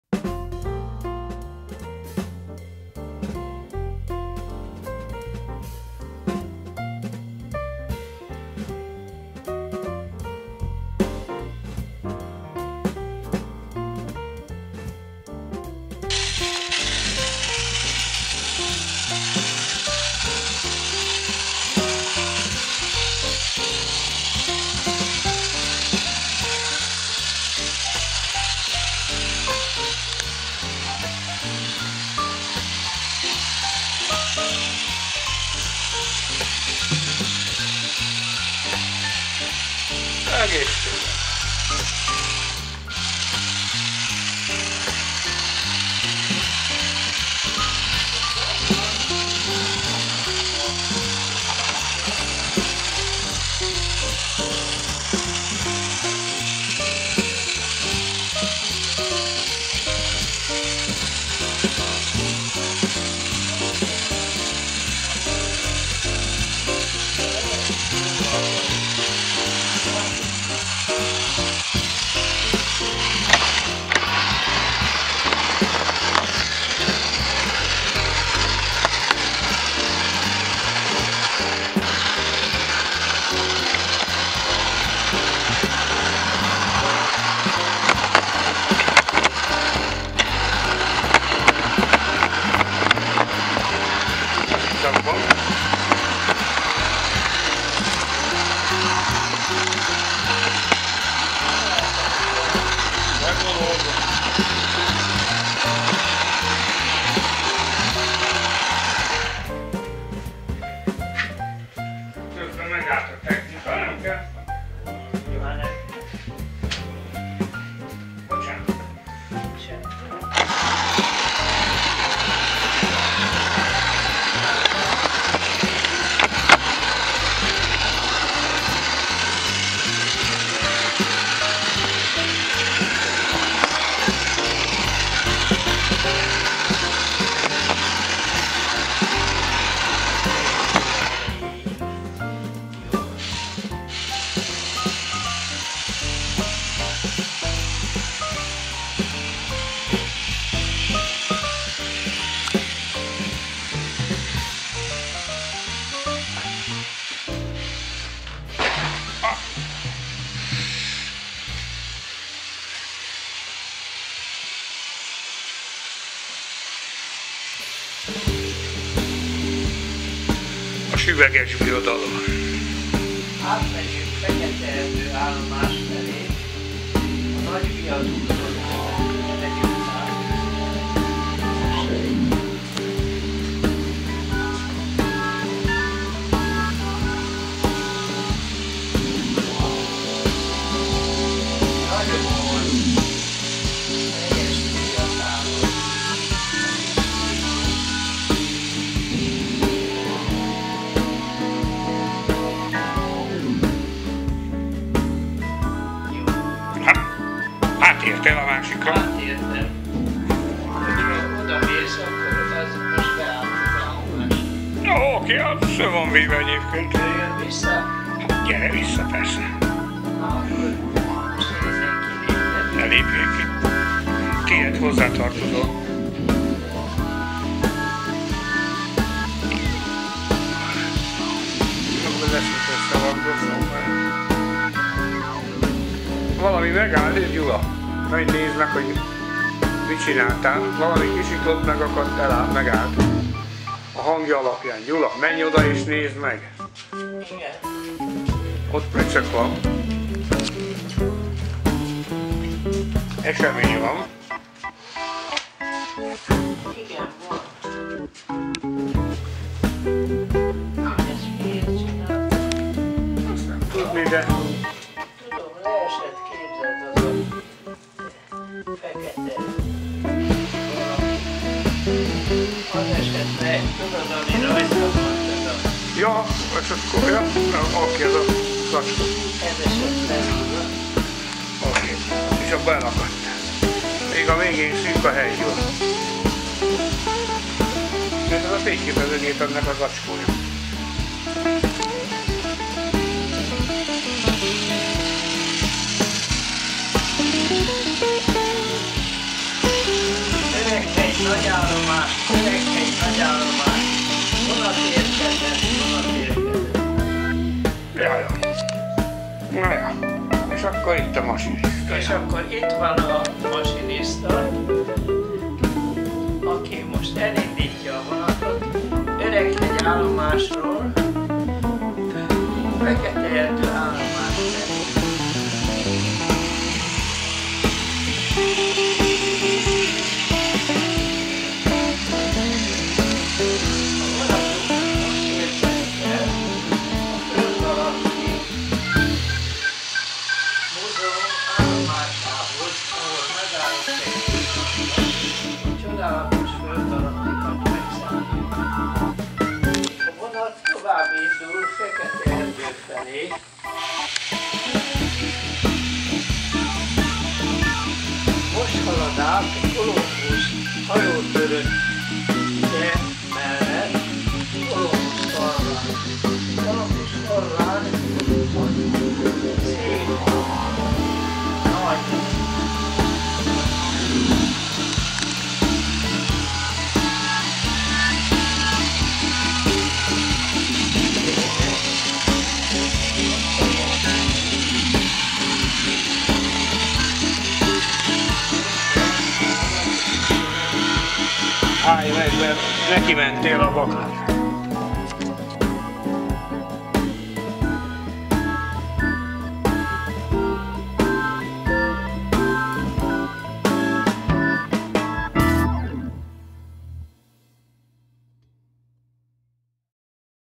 R.K.V.t.
Gyula  barátom még mindíg  keméyen tolja.   Szeretnék  én is  80 évesen ilyen lelkesen vonatozni.